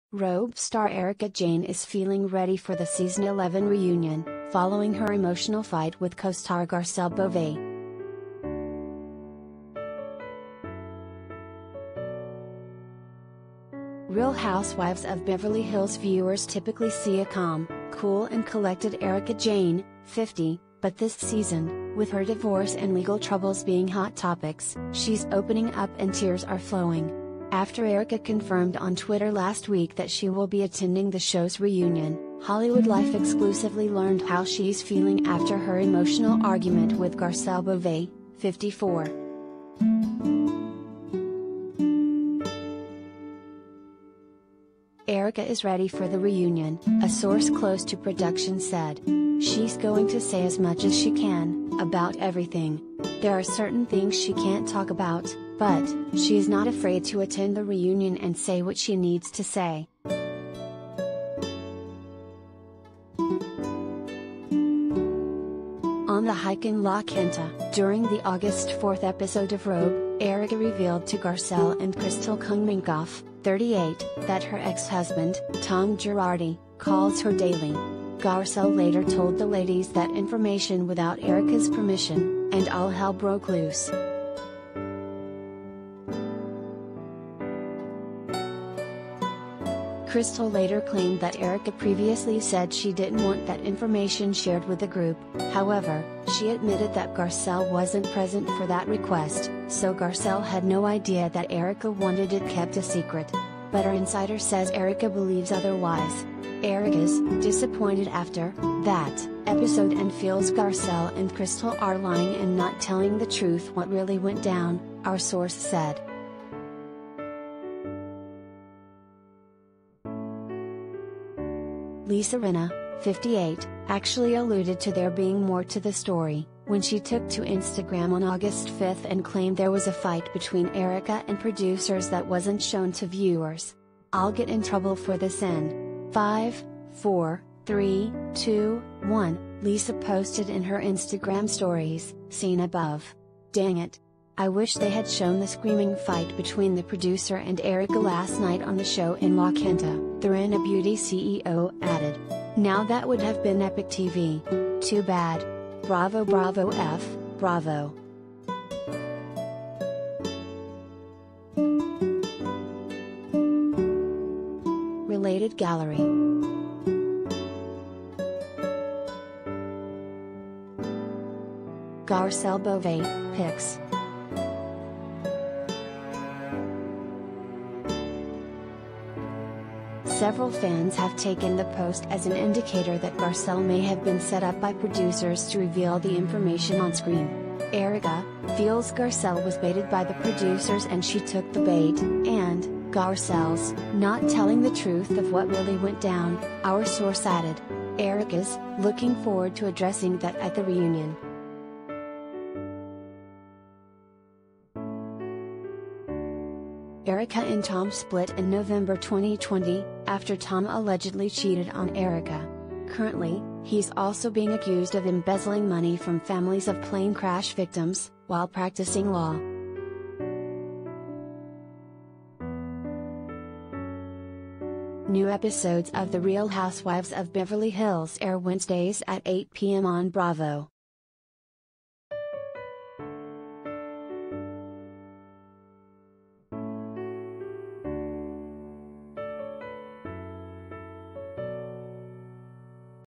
Erika Jayne Is ‘Ready’ For ‘RHOBH’ Reunion After ‘Disappointing’ Fight With Garcelle Beauvais
‘RHOBH’ star Erika Jayne is feeling ‘ready’ for the Season 11 reunion, following her emotional fight with co-star Garcelle Beauvais.
Real Housewives of Beverly Hills viewers typically see a calm, cool and collected Erika Jayne, 50, but this season, with her divorce and legal troubles being hot topics, she’s opening up and tears are flowing. After Erika confirmed on Twitter last week that she will be attending the show’s reunion, HollywoodLife EXCLUSIVELY learned how she’s feeling after her emotional argument with Garcelle Beauvais, 54.
“Erika is ready for the reunion,” a source close to production said. “She’s going to say as much as she can [about everything]. There are certain things she can’t talk about, [but] she’s not afraid to attend the reunion and say what she needs to say.”
On a hike in La Quinta — during the Aug. 4 episode of RHOBH — Erika revealed to Garcelle and Crystal Kung Minkoff, 38, that her ex-husband, Tom Girardi, calls her daily. Garcelle later told the ladies